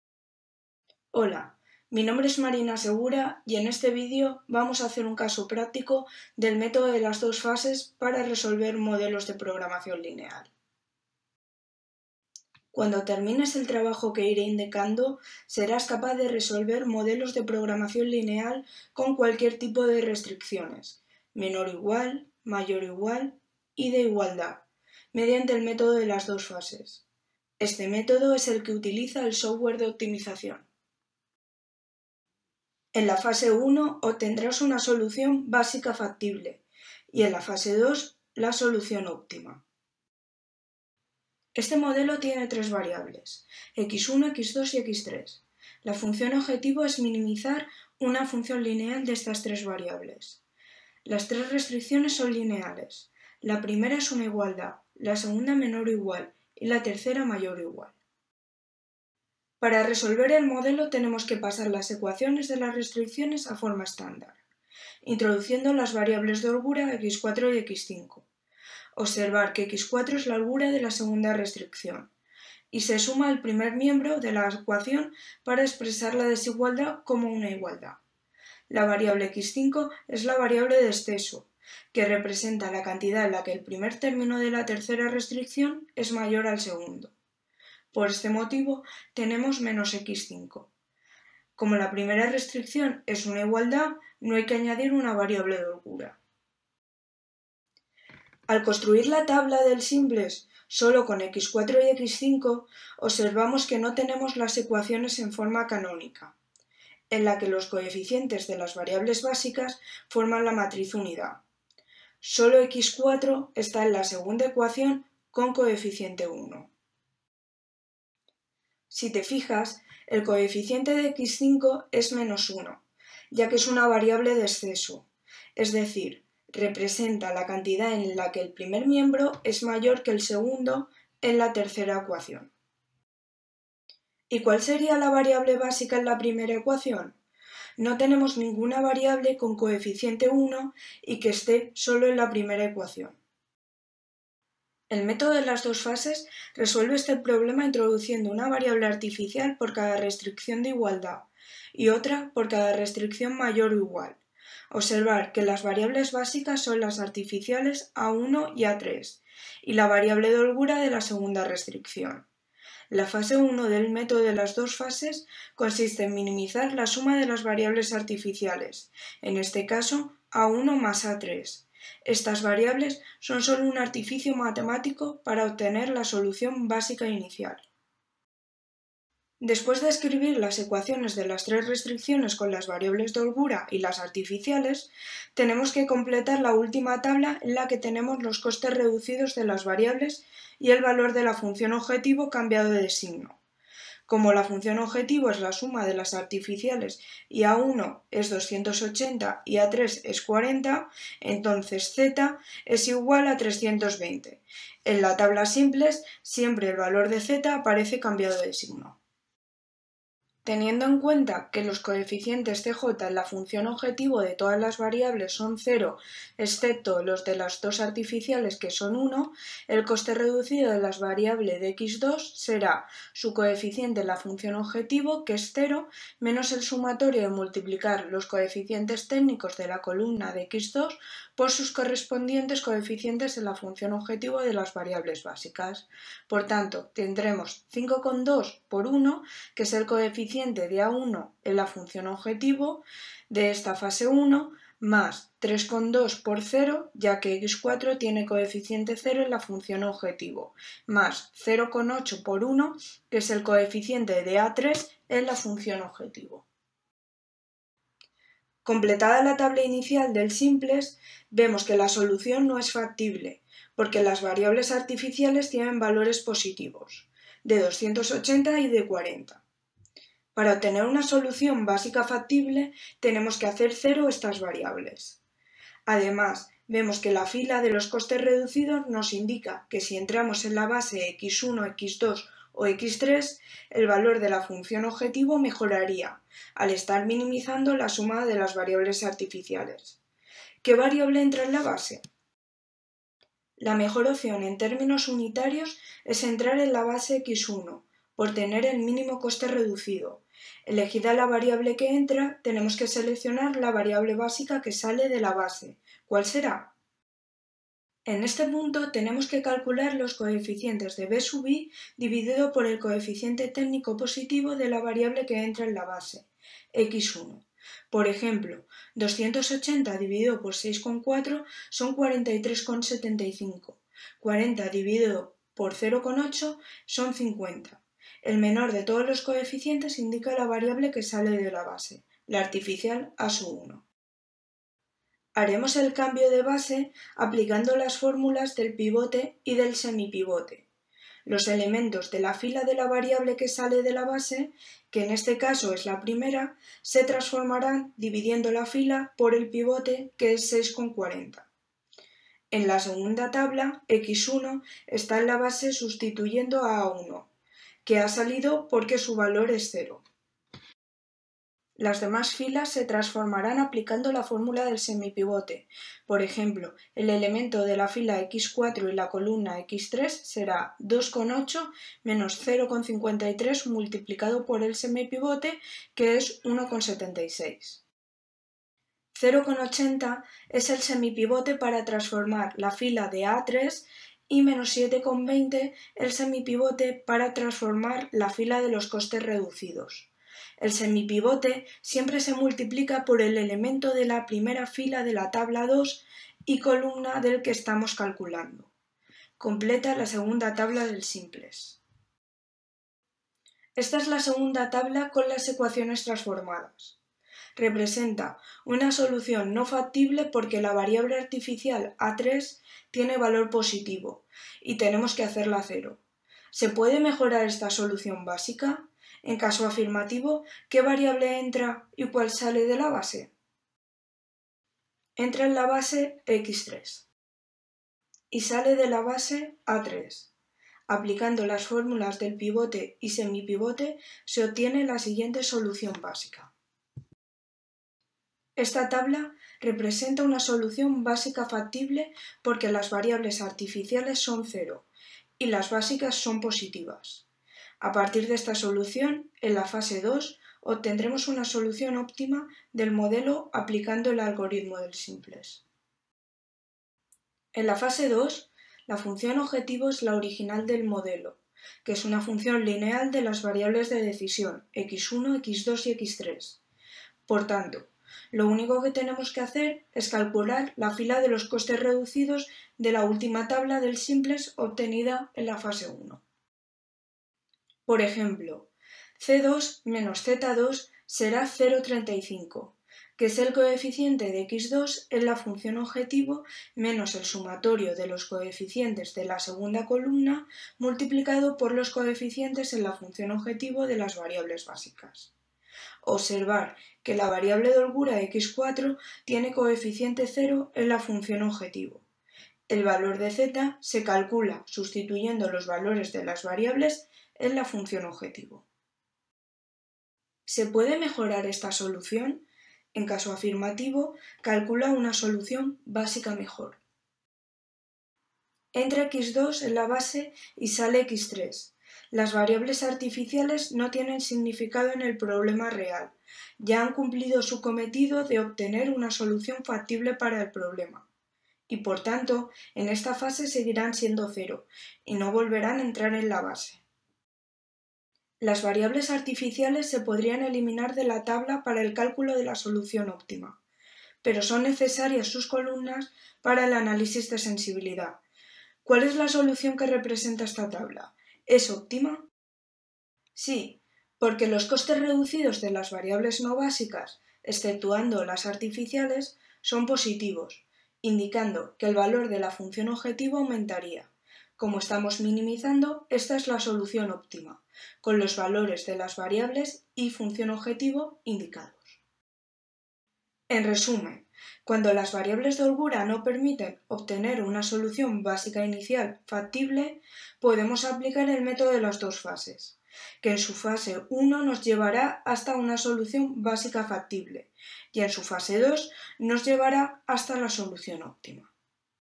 Programación Lineal. Algoritmo del Simplex. Método de las dos fases| UPV
Título: Programación Lineal. Algoritmo del Simplex. Método de las dos fases

Descripción: Caso práctico del método de las dos fases para resolver modelos de programación lineal con cualquier tipo de restricciones, menor o igual, mayor o igual y de igualdad, mediante el método de las dos fases. Este método es el que utiliza el software de optimización. Segura Maroto, M. (2018). Programación Lineal. Algoritmo del Simplex. Método de las dos fases.